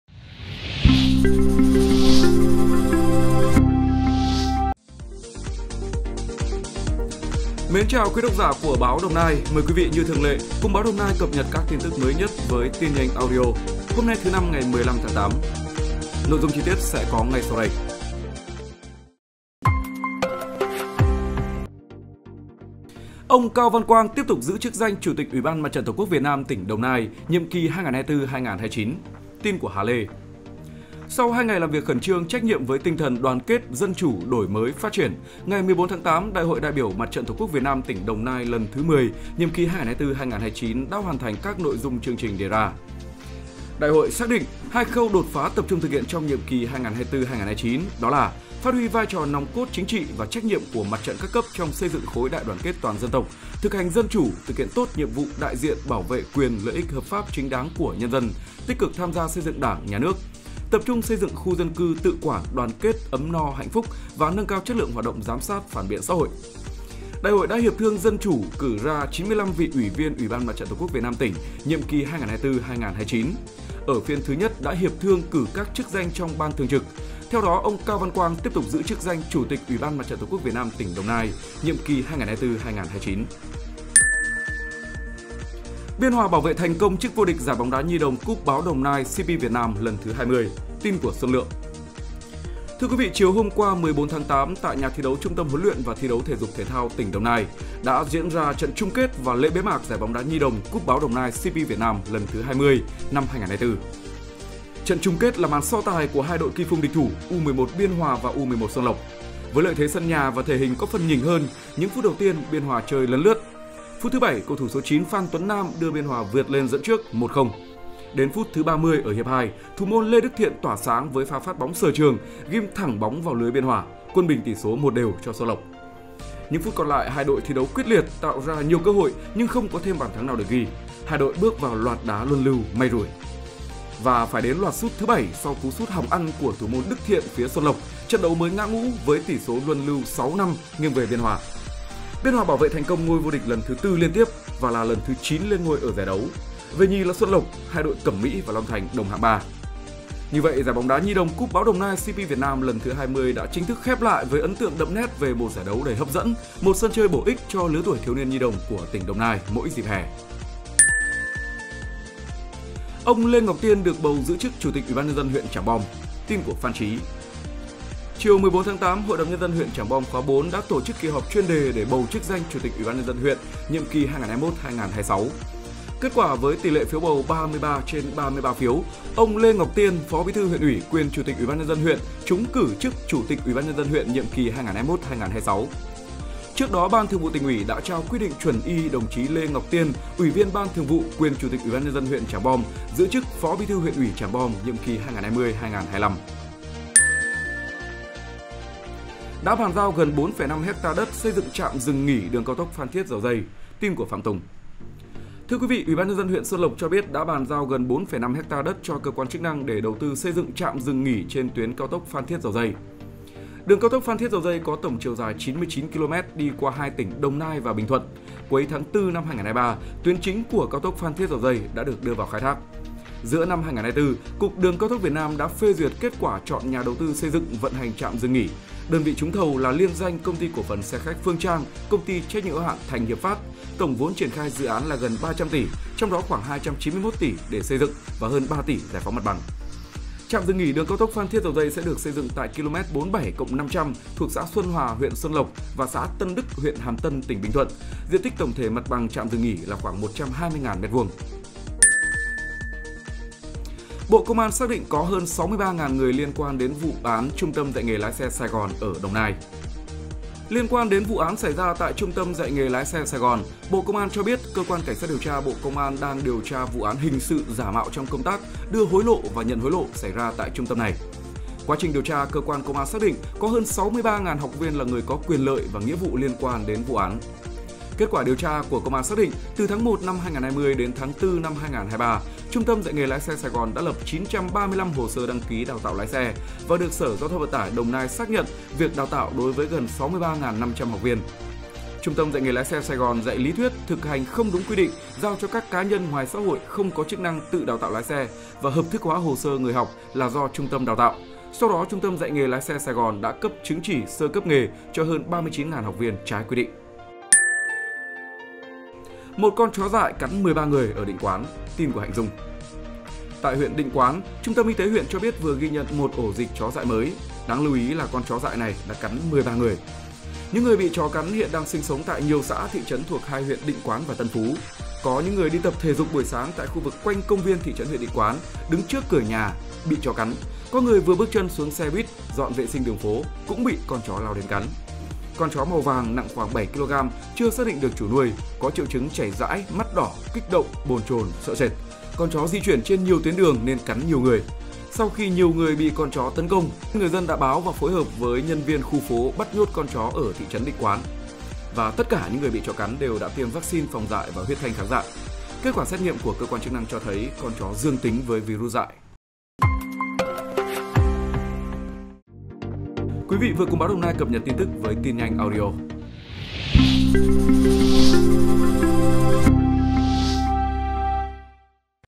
PodCast 15-8-2024
Tin nhanh audio 15-8: Ông Cao Văn Quang tiếp tục giữ chức danh Chủ tịch Ủy ban MTTQ Việt Nam tỉnh Đồng Nai nhiệm kỳ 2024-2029; Biên Hòa bảo vệ thành công chức vô địch Giải bóng đá nhi đồng Cúp Báo Đồng Nai – C.P. Việt Nam lần thứ 20;